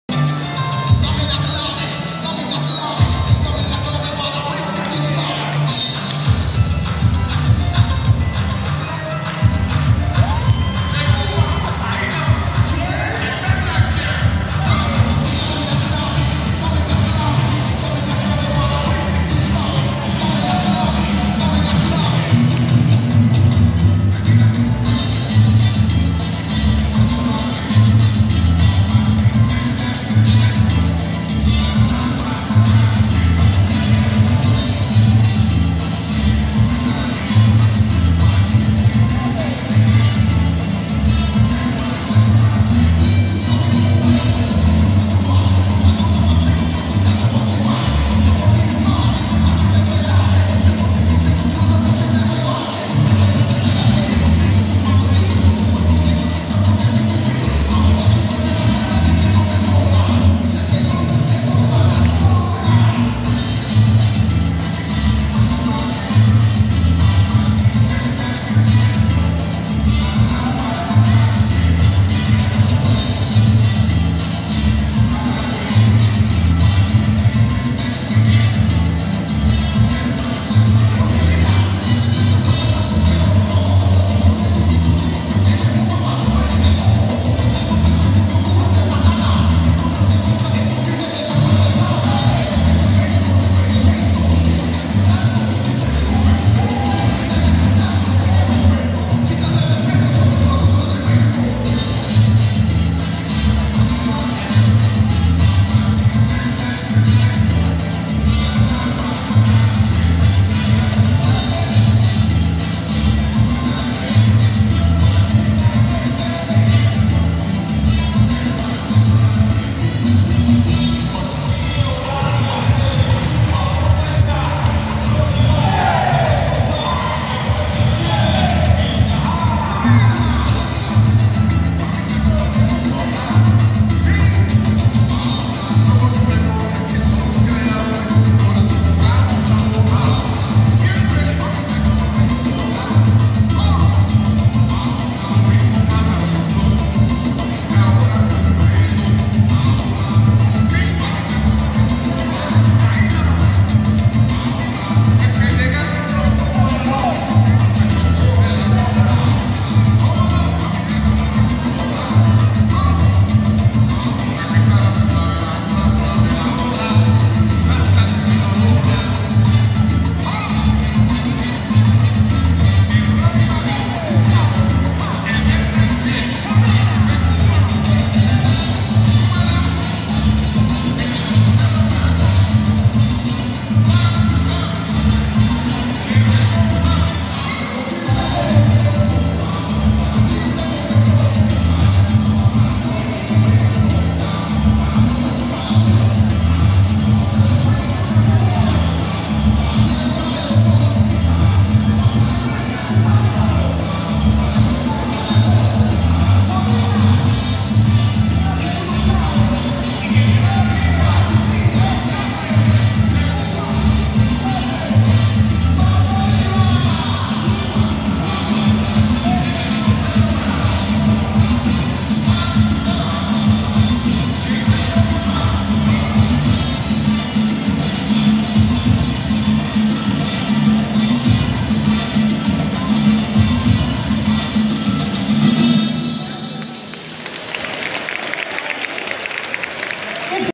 Mojitos - Salsa AUTH - TDC 2023
Mojitos Rueda De Casino team from Salsa AUTH, only contestants in TDC 2023.

Vasilis Giannoglou
Vasilis Chatzigeorgiou
Vasilis Koulakidis
Vaggelis Spiliotis
Thanos Doublis
Stelios Touristas
Suzana Blesiou

Gesthimani Stroumbouli
Ioanna Metallinou
Eva Kartali
Chara Georgiou
Despoina Troullou
Maria Papanikolaou
Marilena Konstantinidi